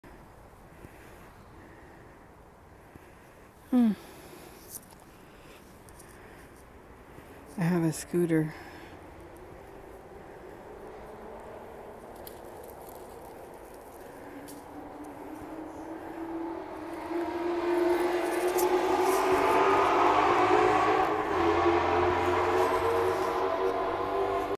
1/2/21 Gangstalkers on electronic scooters on Taft Ave
Not too far from Grant Elementary school and by 101 freeway.